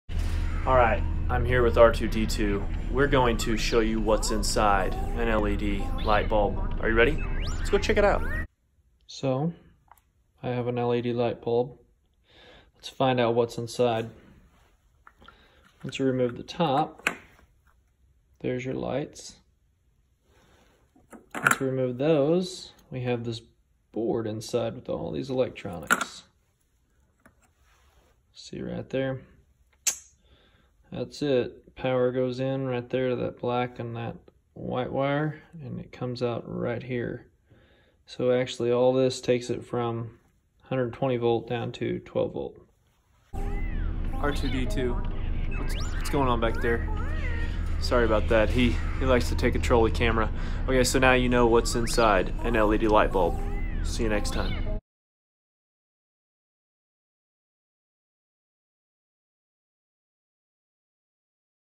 Inside the LED Light bulb
In A Galaxy Far Far Away. We see.. What’s inside a LED Light Bulb